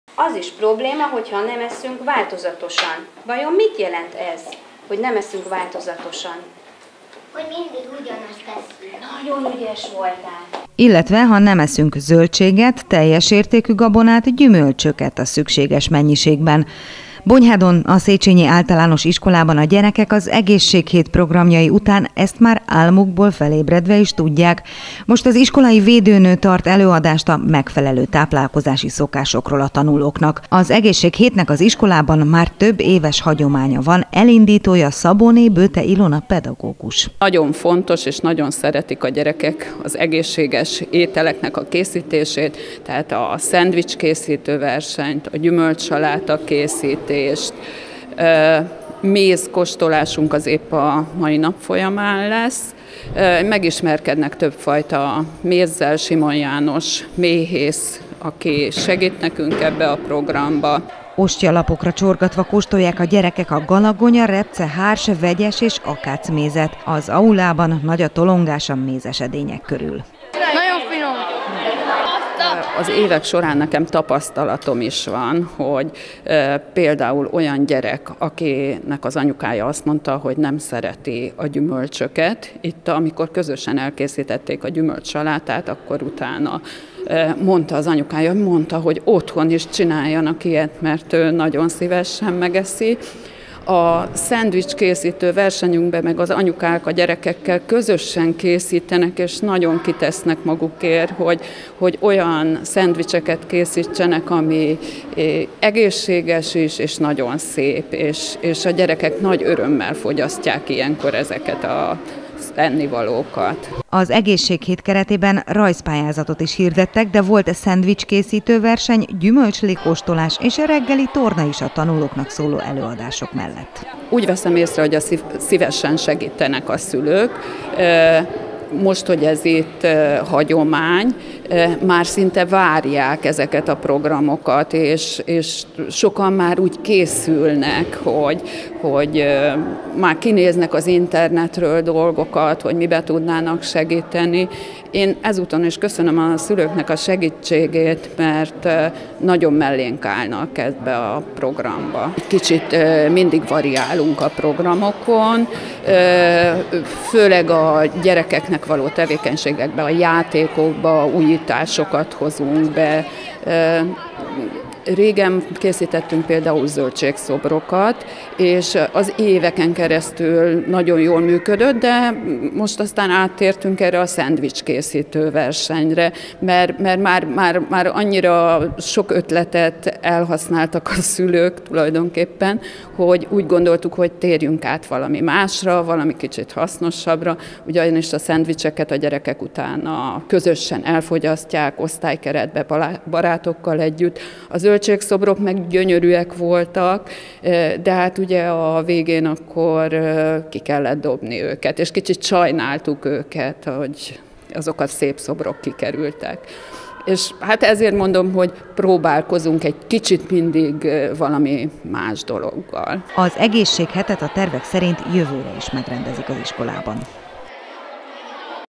Egészséghét Bonyhád 20181019
Reggeli torna, egészséges szendvicsek készítése, gyümölcslé és mézkóstolás, előadások a táplálkozás és megfelelő mozgásformák témákban- mindez Bonyhádon a Széchenyi általános Iskola egészséghetének keretében. A mára már hagyománnyá vált programsorozat évekkel ezelőtt egy pályázat kapcsán indult, azóta már önállóan működik és az éves eseménynaptár kiemelt hete az intézményben.